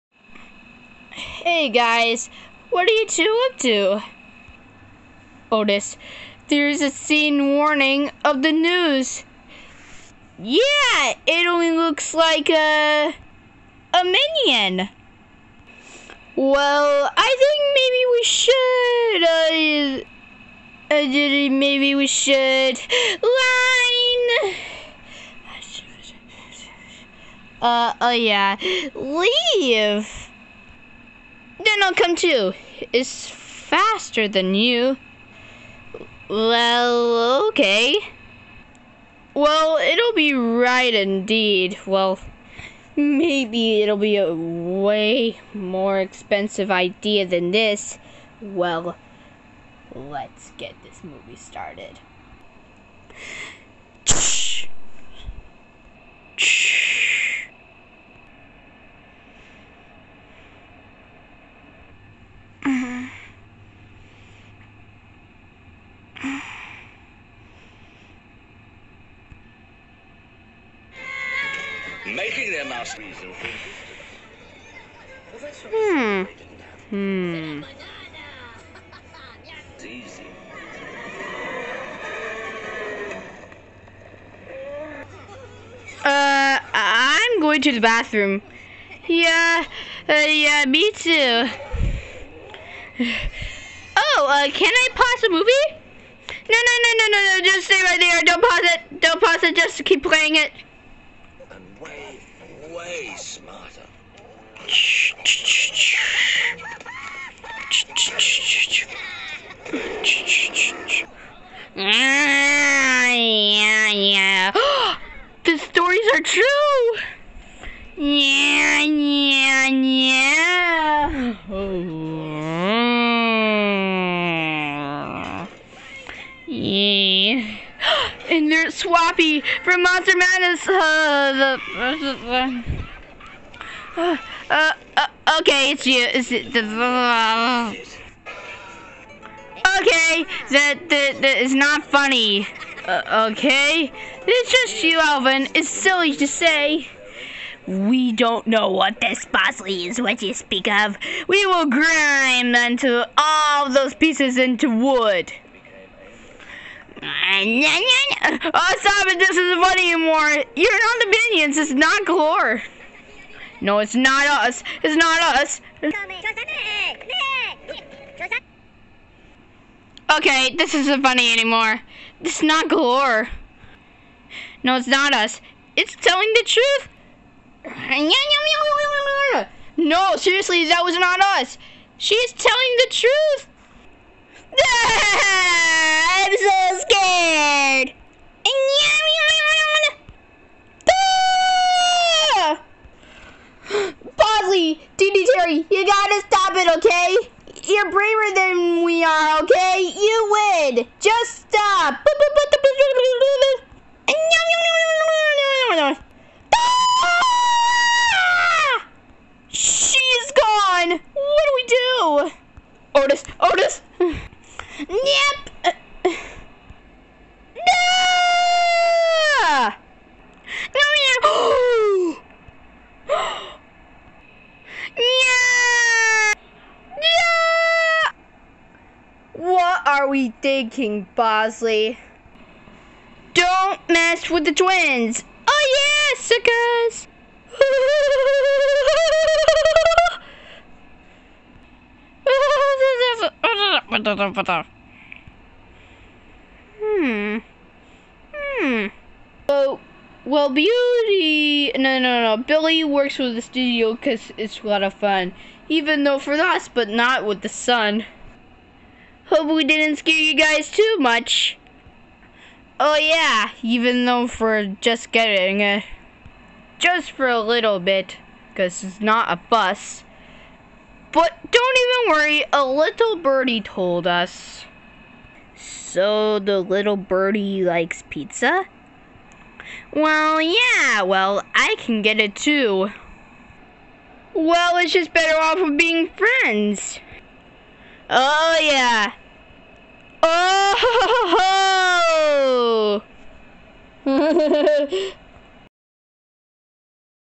The Minions And The Fuzzlies Movie
Minions are taking over the house! But it is just Bozzly and Teeny Terry. But the noise have heard. It is the minions! But they are not noticing. Will they save the room or will it be too late?